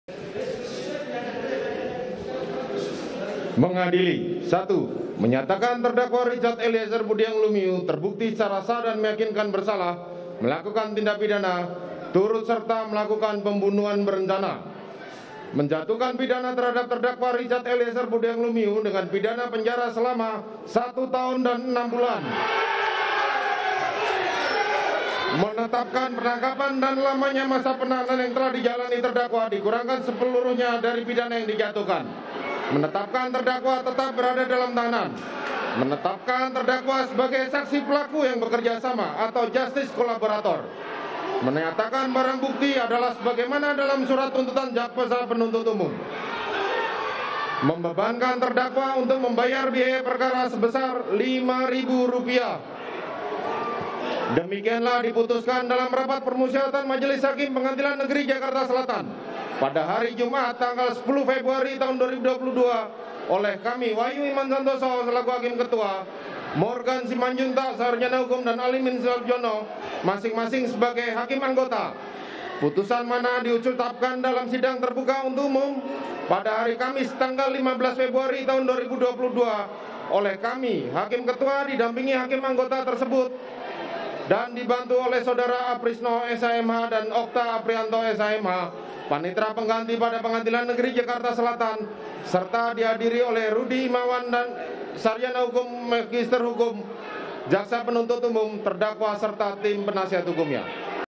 Orang Tua Richard Eliezer Bersimpuh Hingga Sujud Usai Mendengar Vonis yang Dijatuhkan Hakim
JAKARTA, KOMPAS.TV - Usai mendengar vonis yang dijatuhkan Majelis Hakim kepada anaknya, orang tua Richard Eliezer langsung merayakan nya sambil berpelukan hingga bersimpuh.

Pecah tangis pun tak bisa tertahan. Orang tua Eliezer pun bersujud, sebagai tanda syukur atas vonis 1 tahun 6 bulan penjara yang dijatuhkan Majelis Hakim.

Vonis ini jauh lebih ringan dari tuntutan Jaksa Penuntut Umum, yaitu 12 tahun penjara.
Sebelumnya, Richard Eliezer menjadi terdakwa terakhir yang akan menghadapi vonis Hakim dalam kasus pembunuhan berencana Brigadir Yosua Hutabarat.

Diketahui orang tua Richard Eliezer memantau langsung berjalan nya sidang melalui televisi dari kediaman pengacara Ronny Talapessy, di kawasan Bintaro.